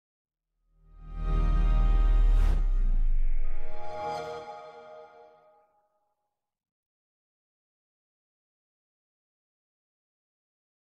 XBOX 360 Start Up (60 FPS)
Going through the closet in my room, I found a XBOX 360 that I used to remember to play a lot until the WiFi adapter inside of it died. There's two other games that I found as well so it should be up momentarily.